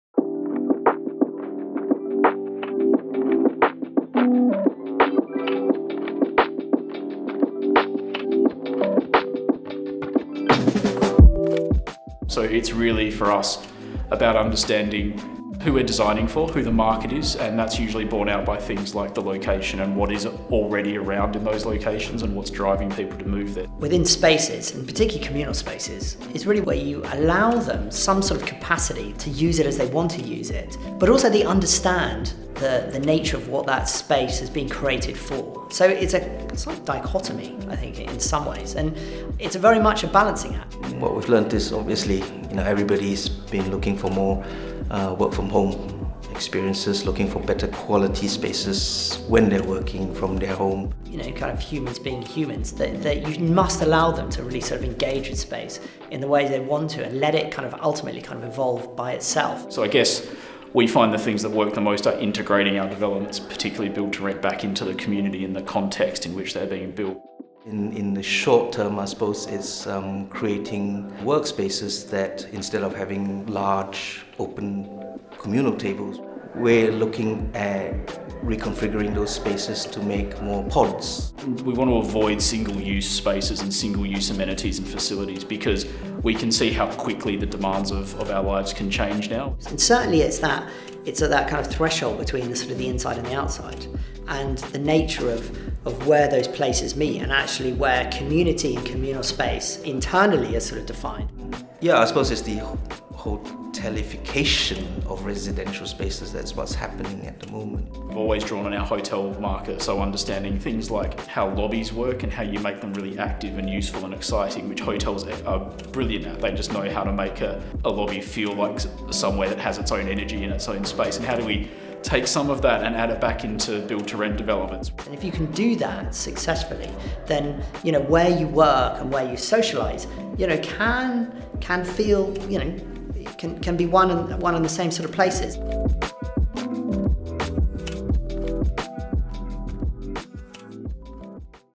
Mix Roundtable: The post-pandemic recipe for Build to Rent success
What has been learned from the pandemic? And what has still to be learned, as BTR prepares for a surge of post-pandemic, investor-driven growth?

A panel embracing BTR developer Quintain, designers including Conran and Partners and Dexter Moren Associates, and the brightest names in the sector including Assael, Tigg & Coll, HTA Design and Woods Bagot, met to judge the past, and chart the future.